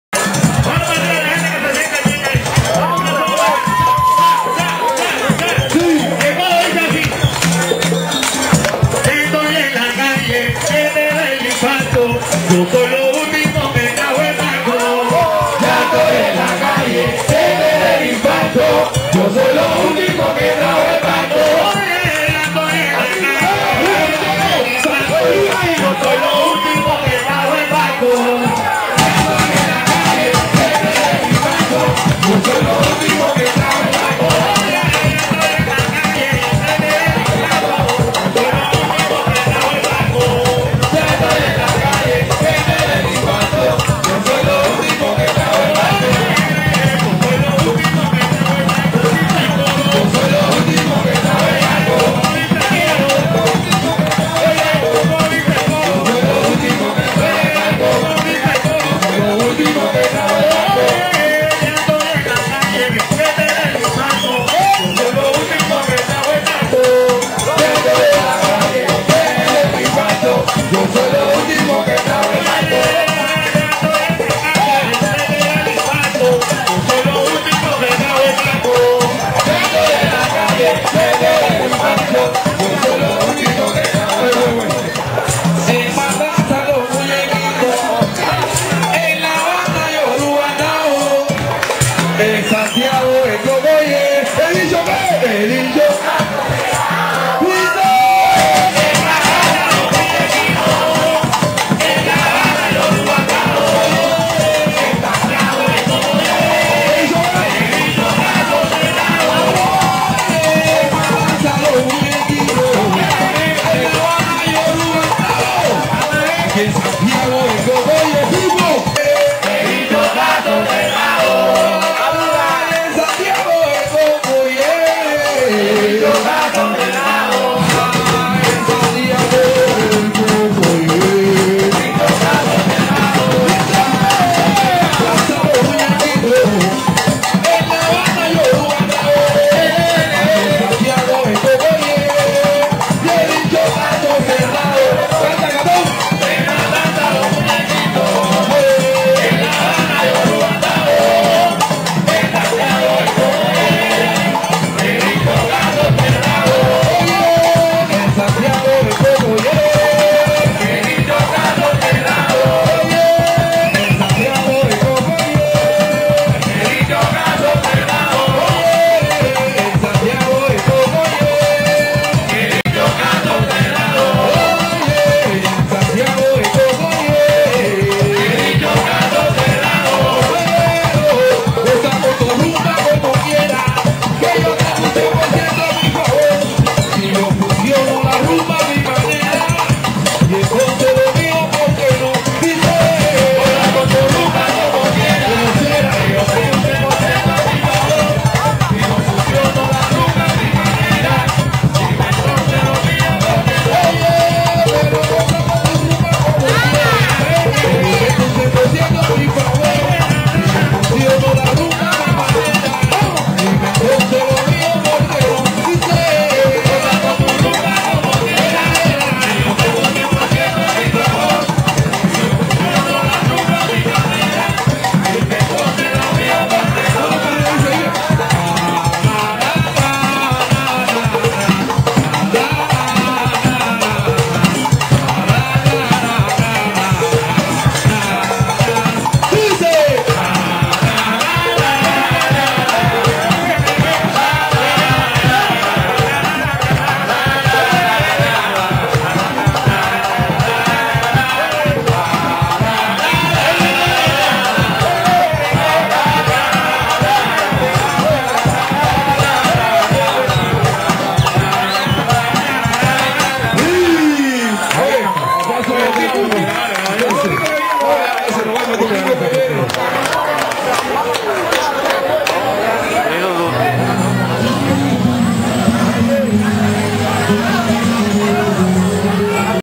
Rumba with Cocoye
Powerful Rumba group based in Santiago de Cuba. Here they combine rumba with the Santiago Conga carnival rhythms. Filmed in the Casa del Caribe, Santiago.